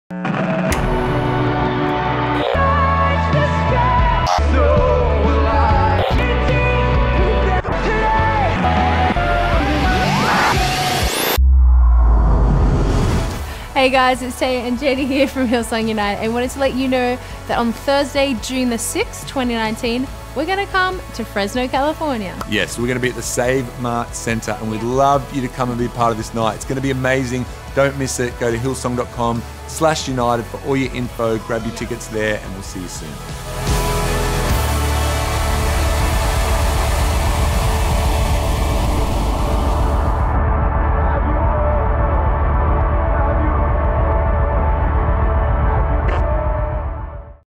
Hillsong UNITED is bringing their 2019 USA Tour to Fresno, CA on June 6th!
Catch Hillsong UNITED for their 2019 USA Tour on June 6th in Fresno, California at the Save Mart Center. New music + a whole new experience.